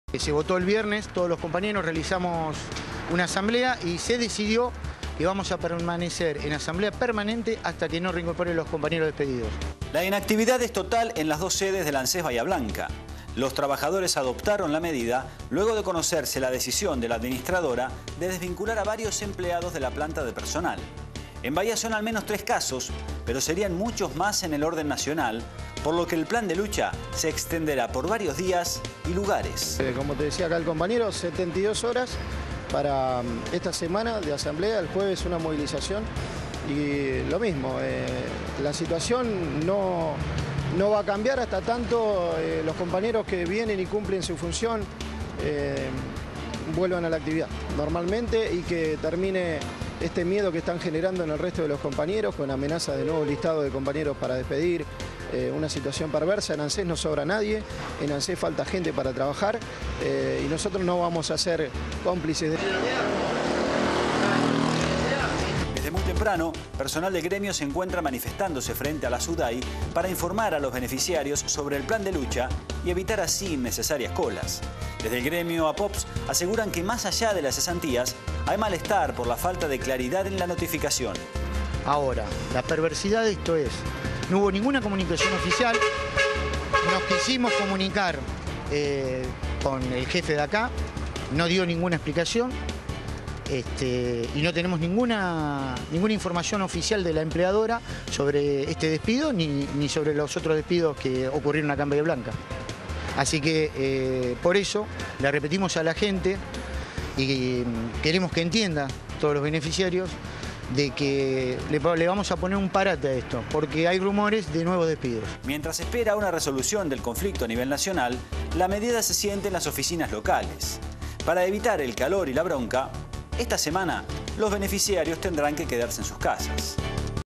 Canal 9 Bahía Blanca - CERRADO POR DESPIDOS
Gremios que nuclean trabajadores de Anses realizan medidas de fuerza por el despido de varios trabajadores en el país. En Bahía son 3 los empleados cesanteados.